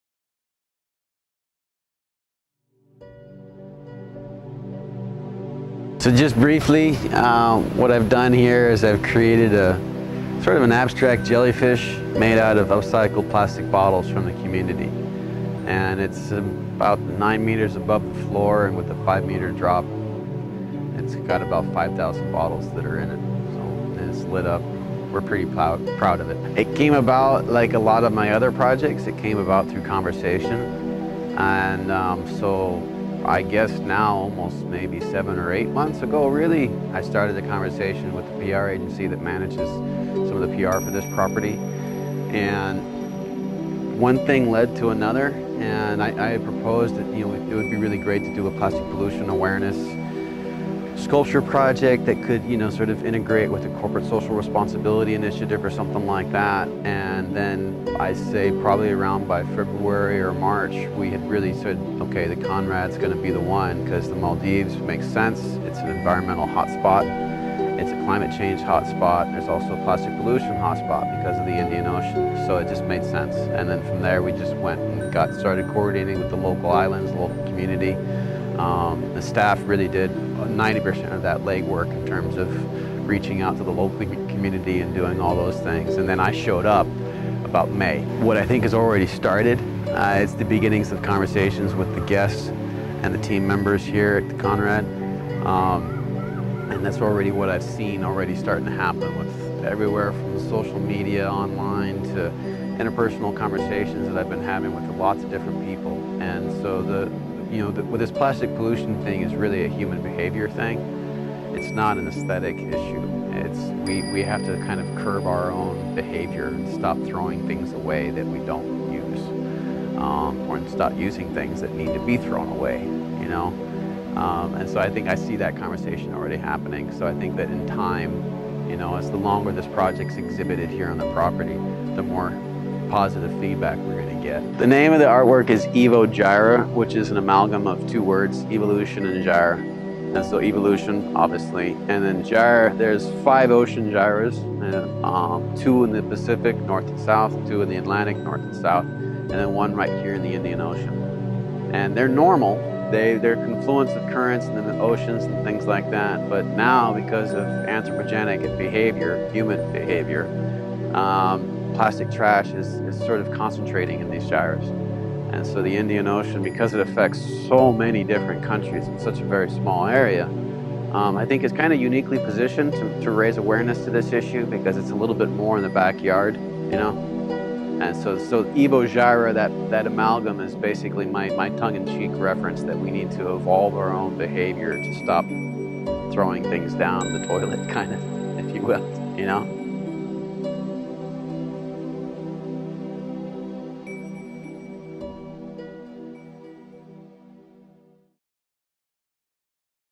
Eco-Art by John K. Melvin | Travel with Purpose
Introducing EvoGyre at ConradMaldives by acclaimed international eco-artist, John K. Melvin. This partnership catalyst to ignite an appreciation for the oceans and the surrounding environment. The large scale installation encourages participation from both the Maldives community and global travelers alike.

EvoGyre is a life-size structure, built at five meters x five meters x seven meters, hangs from the ceiling in Rangali Bar and serves as an educational and interactive touchpoint for guests who find themselves enamored by the piece.

Resembling a jellyfish, a species known for being an indicator of climate change, EvoGyre makes a direct connection between the surrounding marine environment of the Maldives and the impact of plastic. Inspired by THEMURAKA and the surrounding Maldivian community, EvoGyre serves as a visual reminder of the humankind responsibility to protect the oceans.
She has been assembled using nearly 5,500 locally-sourced single-use plastic bottles held together by coir rope, steel cable and illuminated by LED lighting.